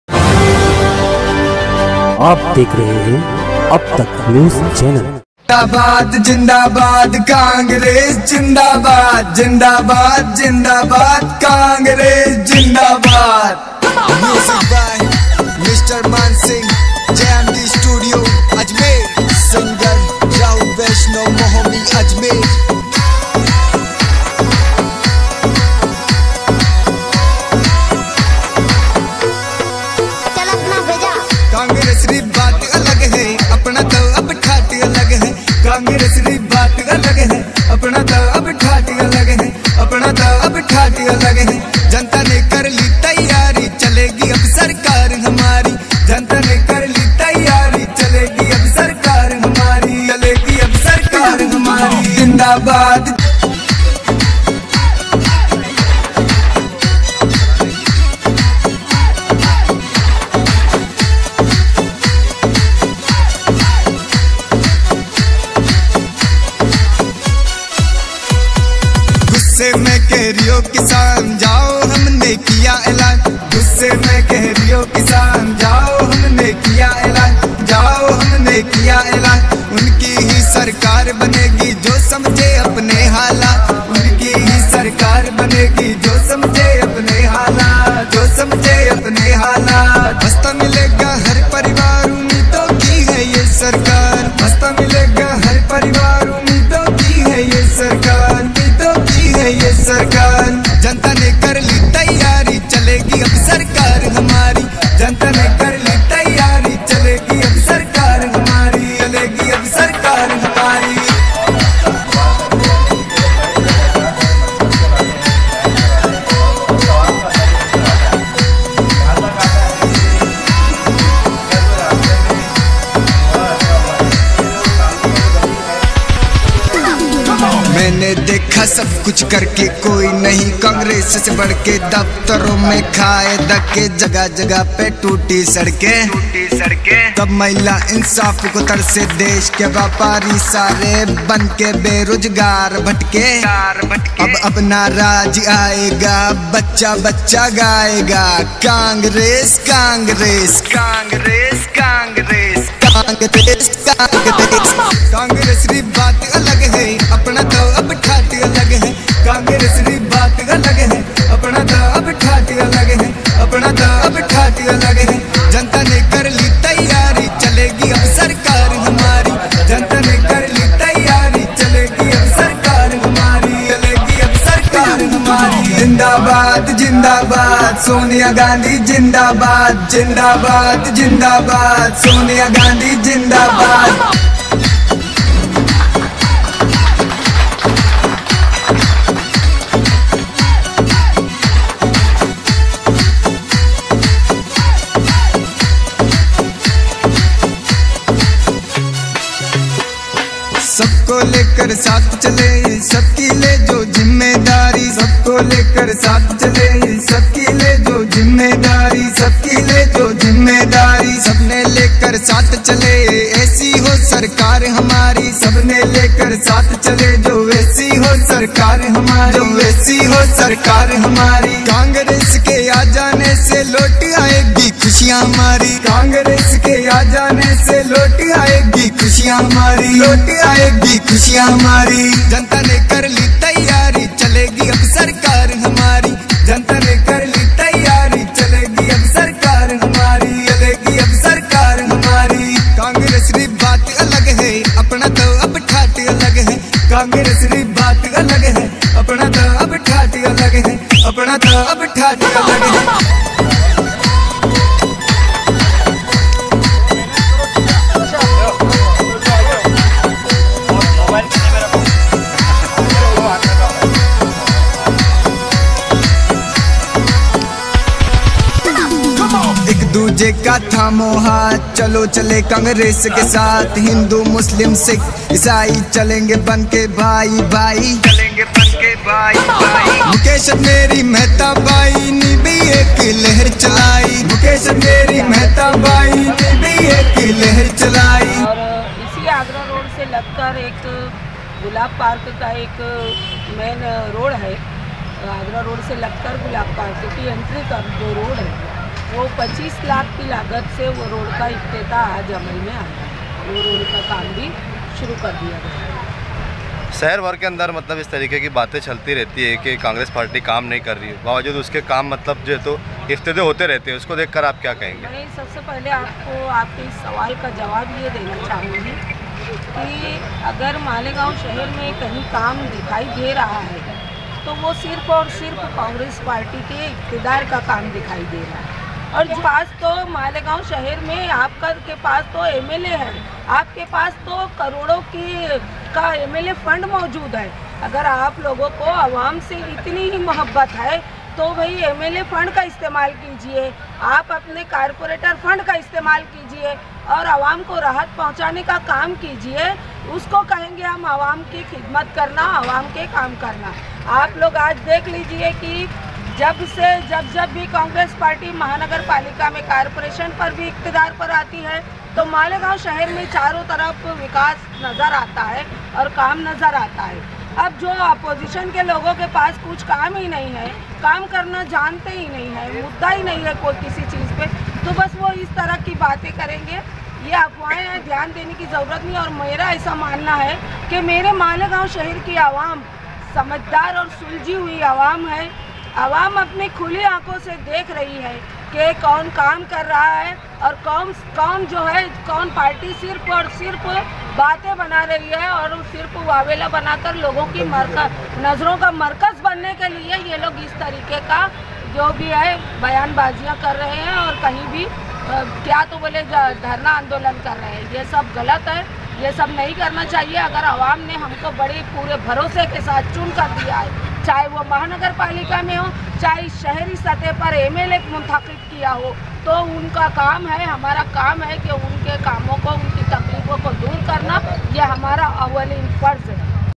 इस शहर में कांग्रेस पार्टी काम करती आई है और काम करती रहेंगी मुखालिफइन मुखालफत करते रहे طاہرہ شیخ
इस शहर में कांग्रेस पार्टी काम करती आई है और काम करती रहेंगी मुखालिफ इन मुखालफत करते रहे طاہرہ شیخ